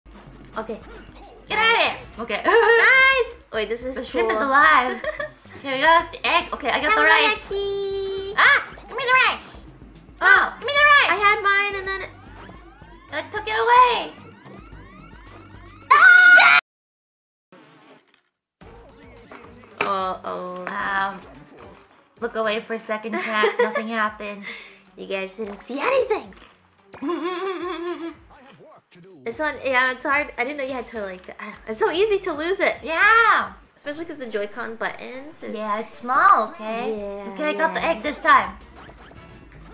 𝘛𝘩𝘪𝘴 𝘨𝘢𝘮𝘦 𝘮𝘢𝘬𝘦𝘴 𝘈𝘮𝘦 𝘴𝘢𝘺 𝘵𝘩𝘦 "𝘚" 𝘞𝘰𝘳𝘥 𝘰𝘶𝘵 𝘭𝘰𝘶𝘥【𝘏𝘰𝘭𝘰𝘭𝘪𝘷𝘦𝘌𝘕】
She say it out loud..
Source video
Title     : 【COLLAB】 TOGETHAAAAA WE SHALL GAME with @WatsonAmelia !!
Hololive Myth & Council
Ch.  @NinomaeInanis
Tag.